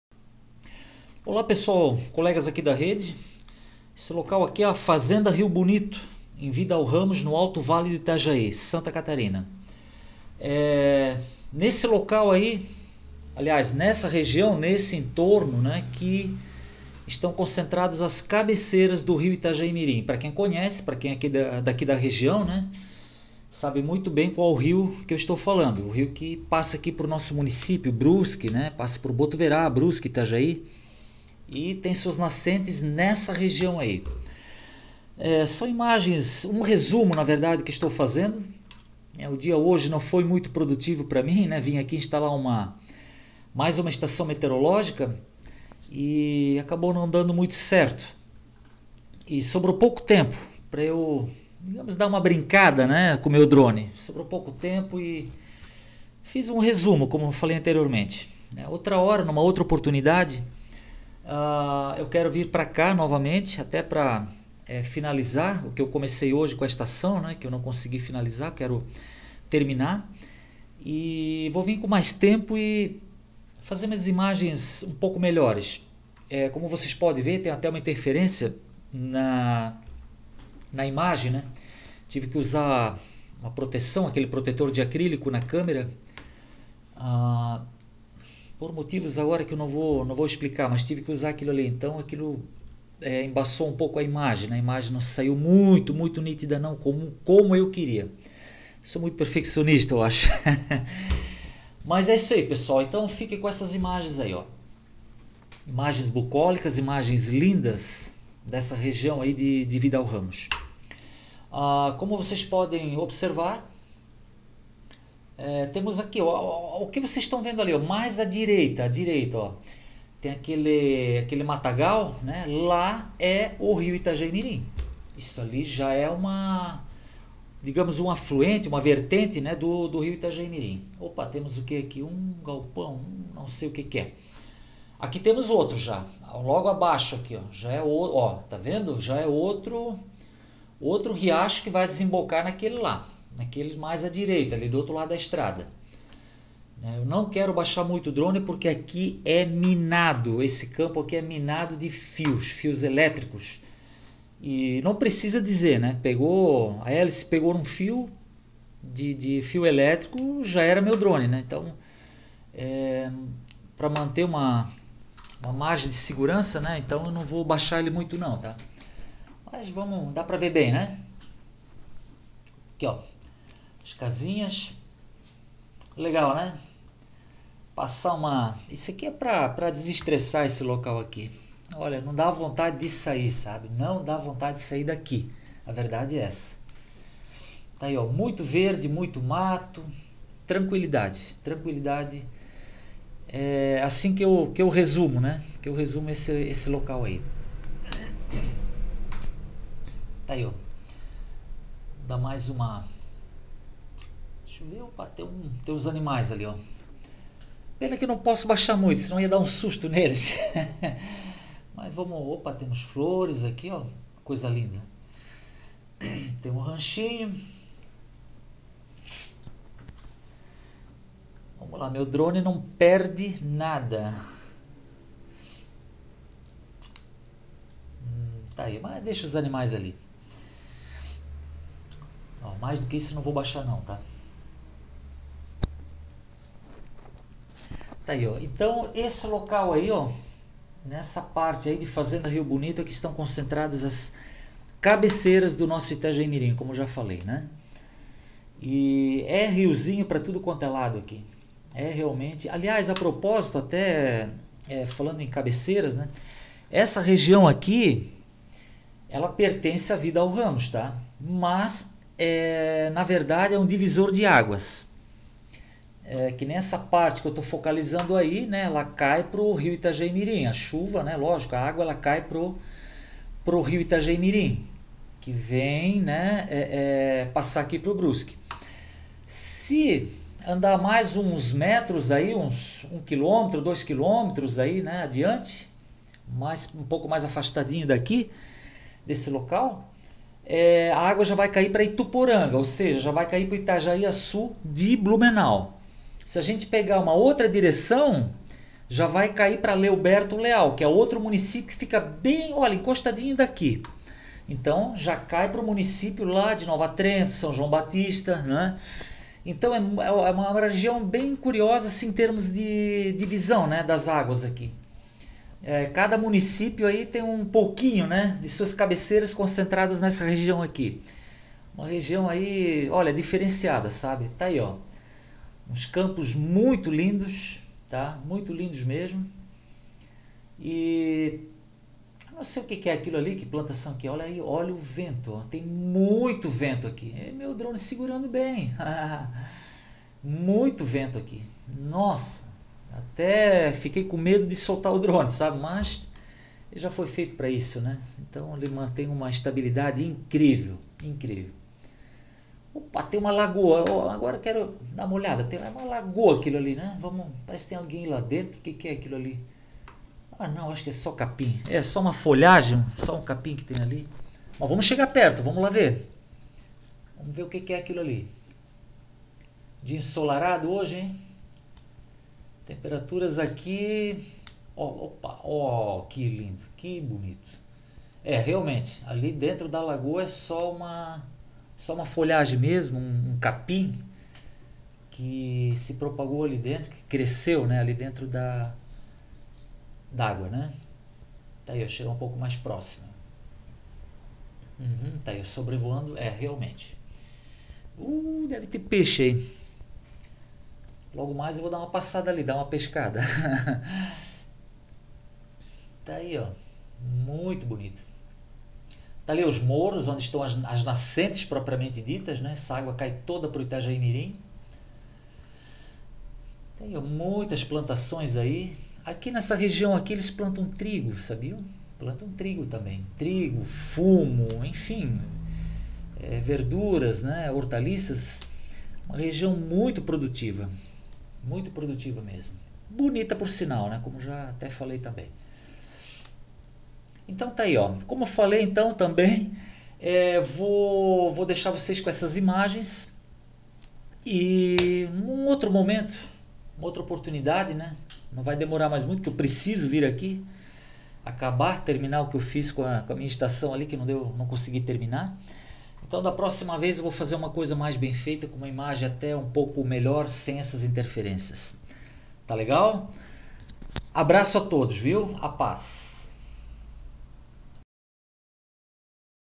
Cabeceiras do Rio Itajaí Mirim
Meu MAVIC PRO sobrevoou a localidade de Fazenda Rio Bonito, em Vidal Ramos-SC.